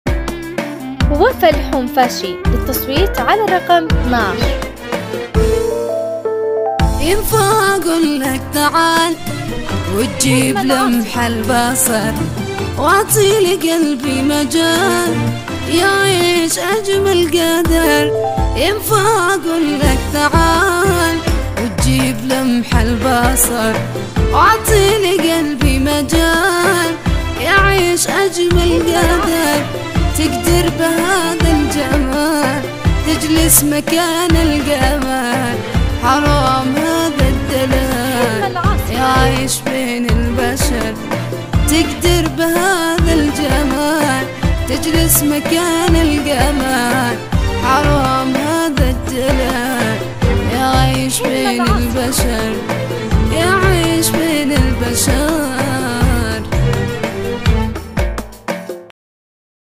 الفنانة وفاء الحنفاشي في المرحلة الاخيرة والحاسمة في منافسة مطربة الموسم 2023 🔥
الفنانة / وفاء الحنفاشي 🔥
في المرحلة الأخيرة والحاسمة
يمكنكم التصويت للفنانة من خلال المنشور الرئيسي المثبت في الصفحة
 بكتابة اسم الفنانة او رقمها التصويتي(12)
منافسة مطربة الموسم مستمرة حتى تاريخ 15مايو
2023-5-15